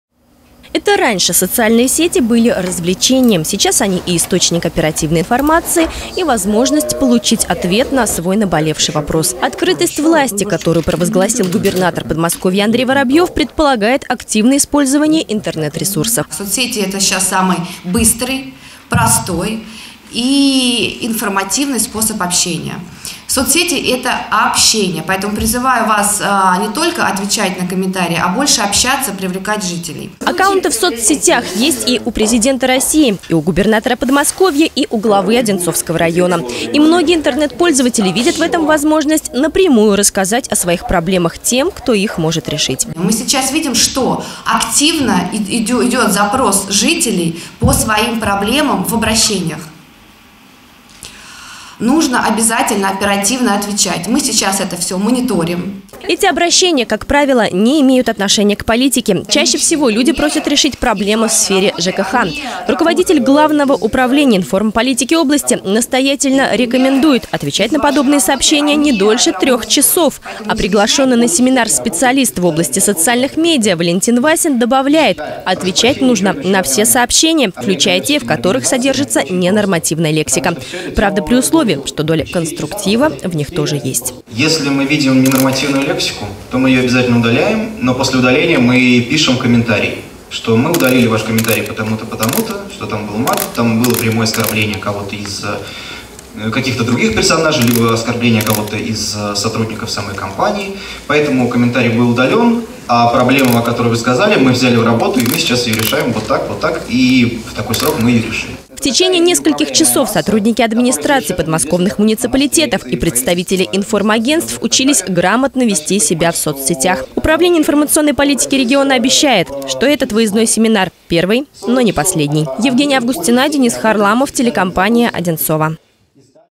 Учебный семинар в администрации Одинцовского района
В  Одинцовском районе прошел первый  выездной семинар для сотрудников администраций муниципальных образований Подмосковья и руководителей региональных информагентств. Организатором выступило Управление информационной политики Московской области.